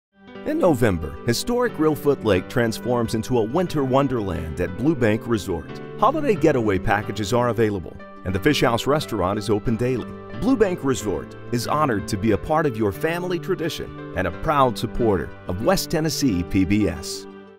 Blue Bank Resort Winter Wonderland
We invite you to come see our 1/2 million Christmas light display on the banks of historic Reelfoot lake.  Walk through the 1/2 acre winter Wonderland that is packed FULL of Holiday Cheer!
Santa will join us every weekend in December,  we will have live Christmas music, SNOW & fire pits going to keep warm! We even have s’mores, Come make Blue Bank Resort part of your Family’s holiday tradition.